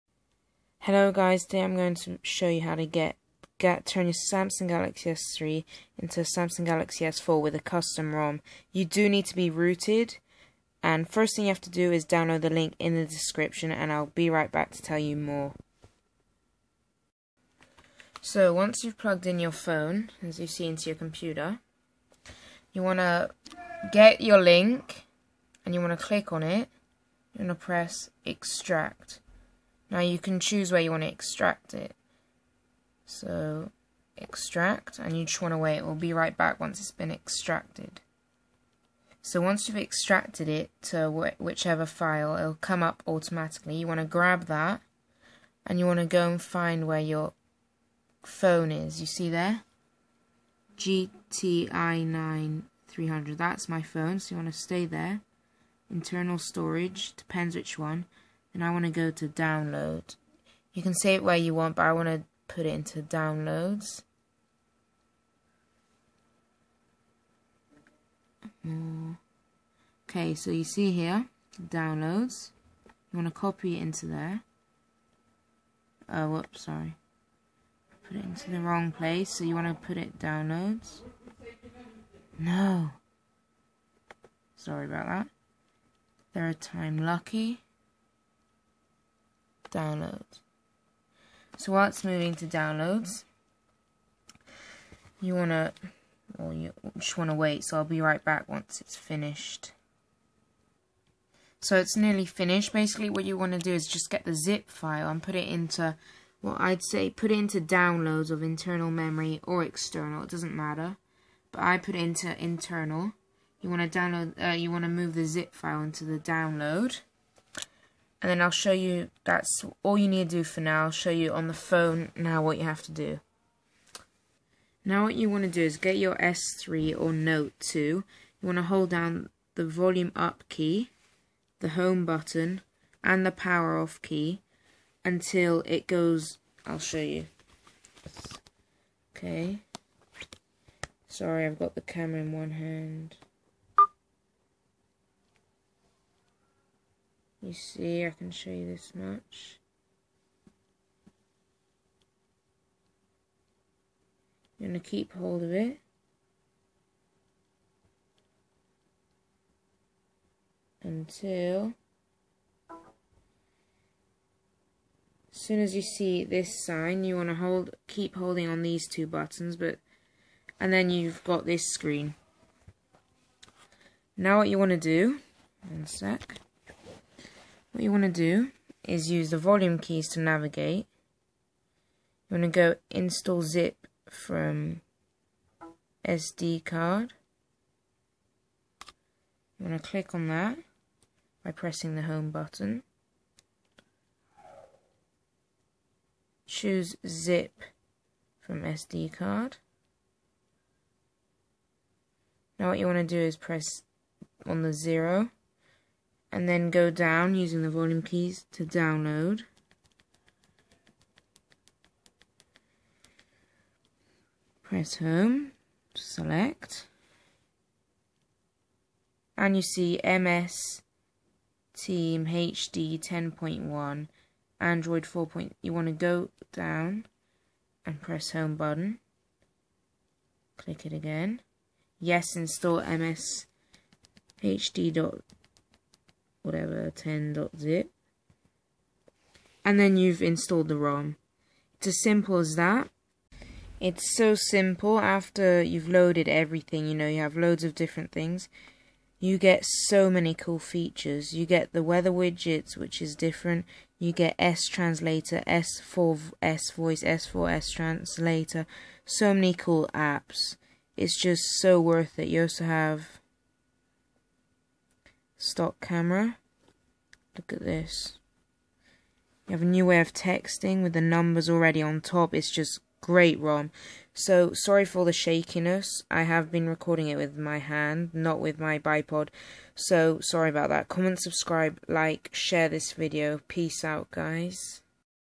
How To Get Galaxy S4 Custom Rom For Galaxy s3
Galaxy s4 custom rom

**ROOT REQUIRED**
**As long as your  S3 is rooted and you have clockworkmod recovery installed, this will work on all firmwares.**